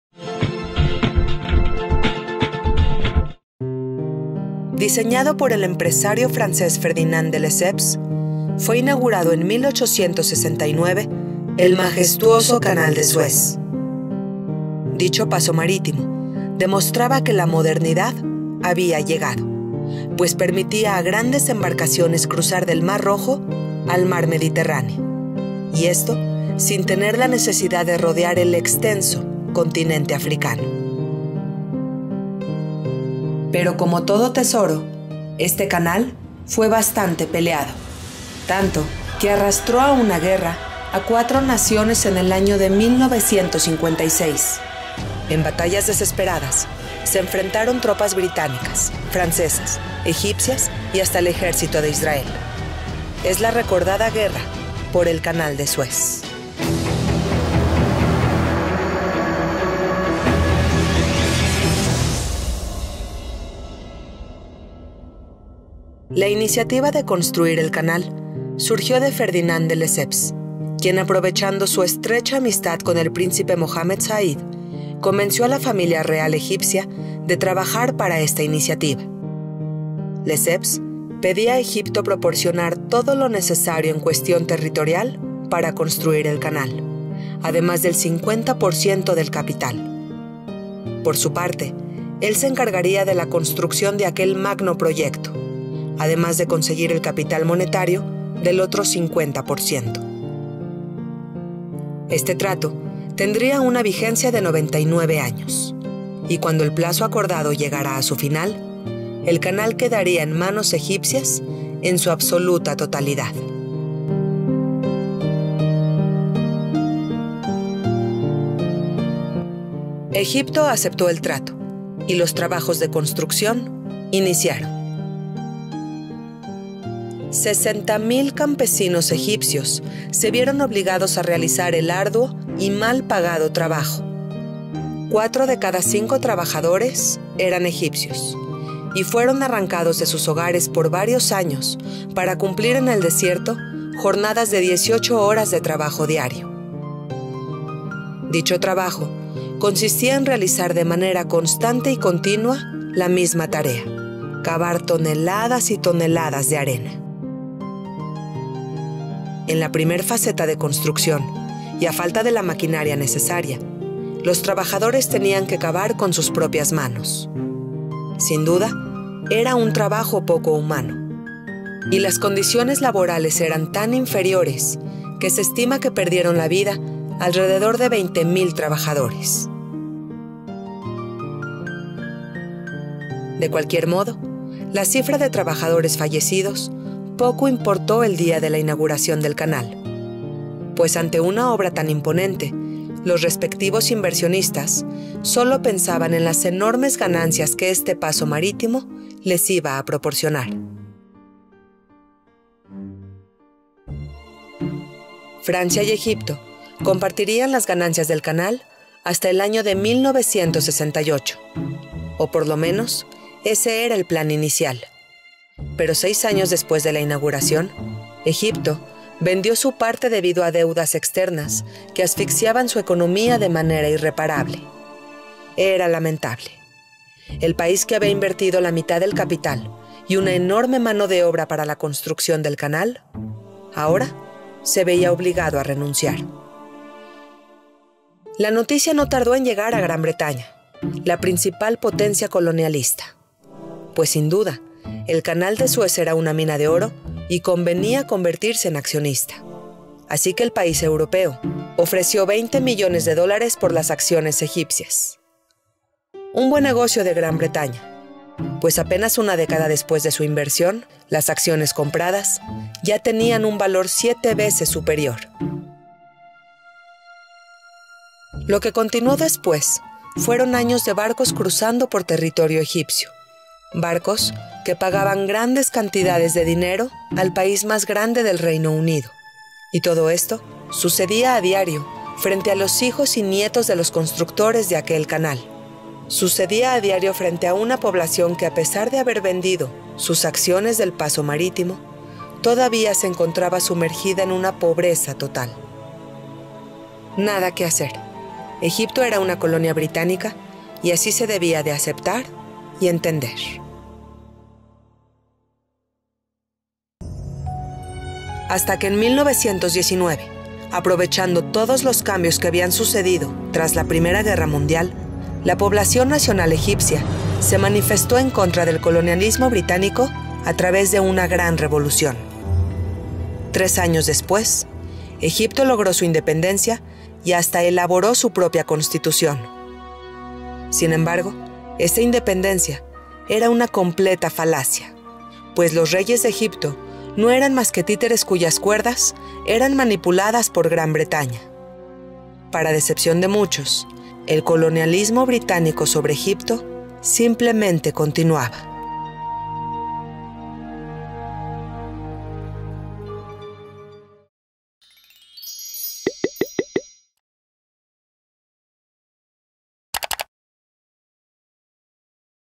¿Quién construyó el Canal de Suez?
Diseñado por el empresario francés Ferdinand de Lesseps, fue inaugurado en 1869 el majestuoso canal de Suez. Dicho paso marítimo demostraba que la modernidad había llegado, pues permitía a grandes embarcaciones cruzar del Mar Rojo al Mar Mediterráneo y esto, sin tener la necesidad de rodear el extenso continente africano.